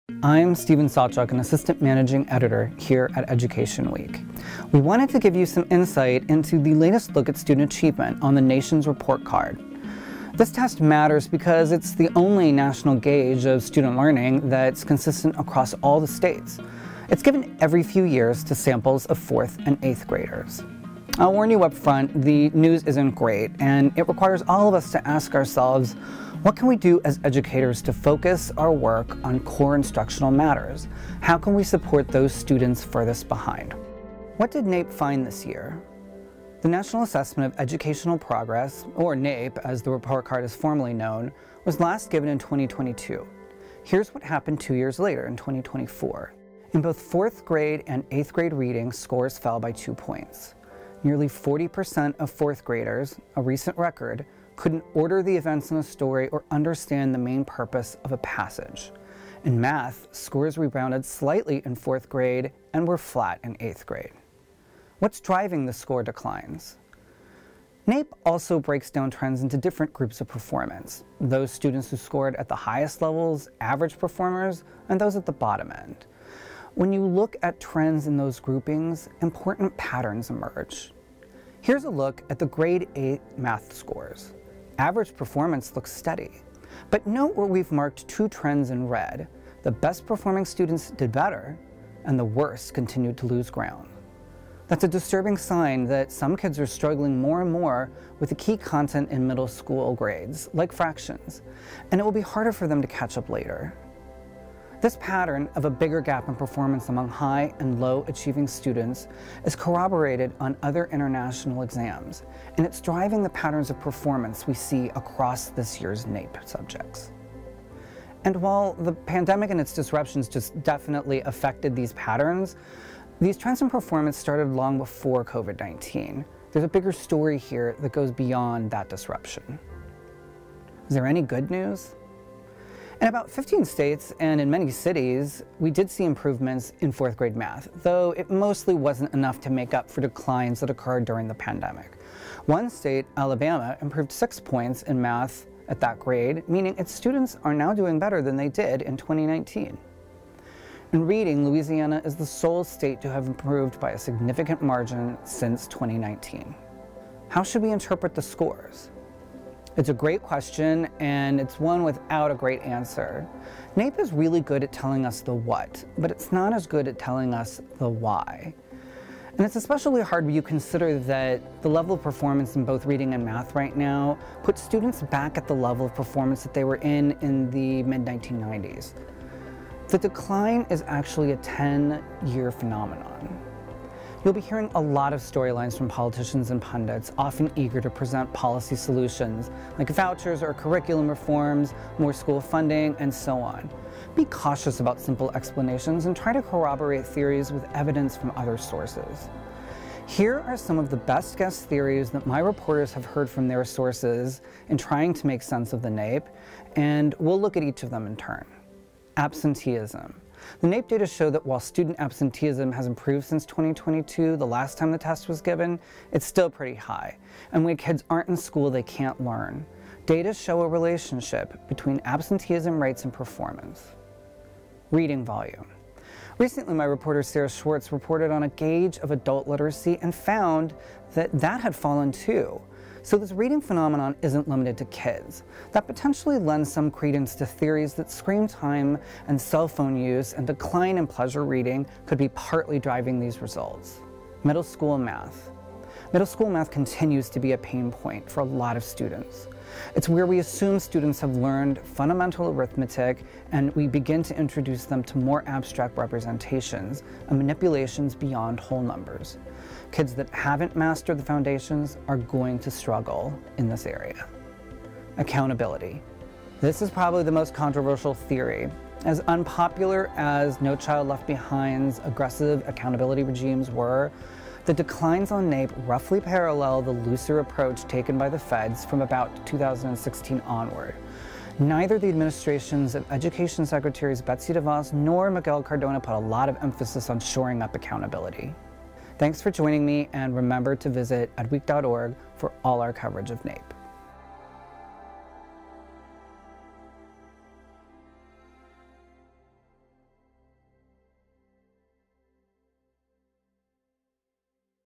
What the Dismal Nation’s Report Card Means for Reading and Math
The latest results of the federally administered National Assessment of Educational Progress (NAEP) show continuing declines in students’ reading scores and some modest progress in math. Listen as Education Week breaks down the findings—and how to interpret them.